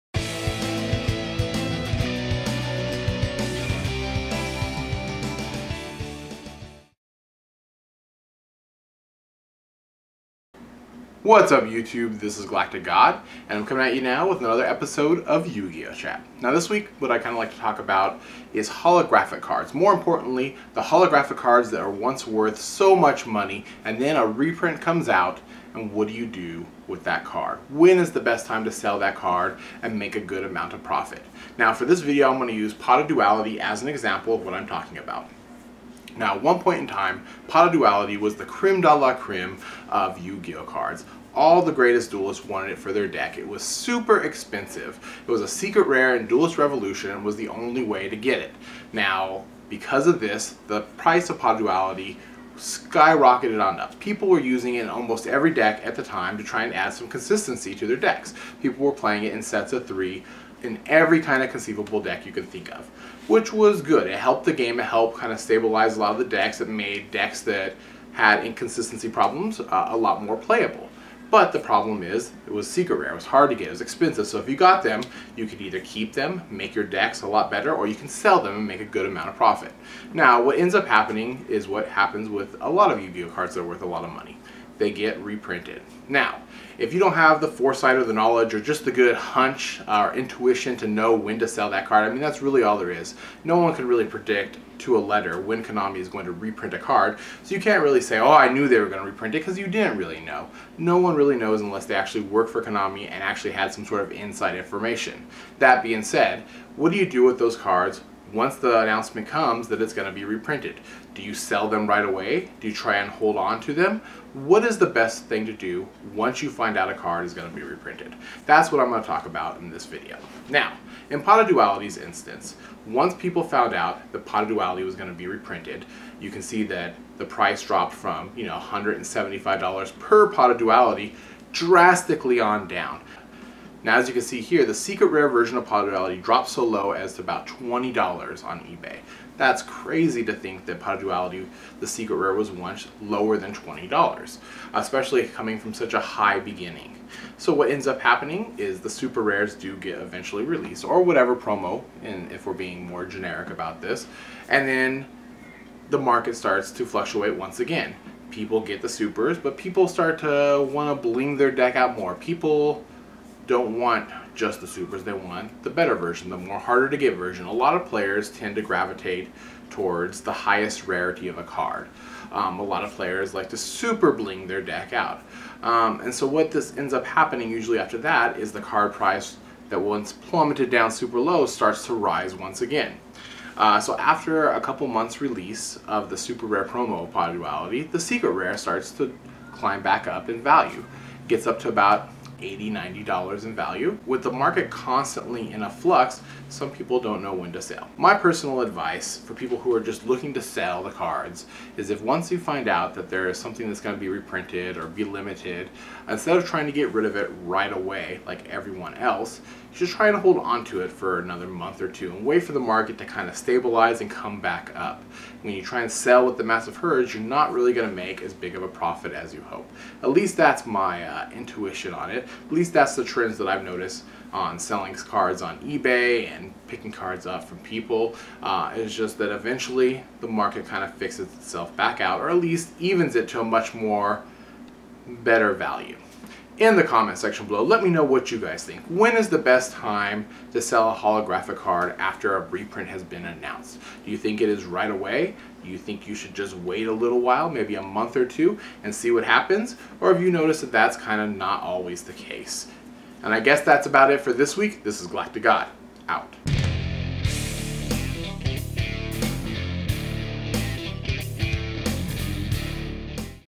Yugioh Chat - Episode 76: How to make a Profit off your Reprinted Card
Episode 76 - How to make a Profit off your Reprinted Card

In this episodes Yugioh Chat, we take a look at valuable cards that get reprinted. What does one do with a card that use to be worth a lot of money, when is the best time to sell such a card and why. In the comment section below, remember to leave me your thoughts on the subject.

How to find me on the Web:

Dueling Network Screen Name: GalacticGod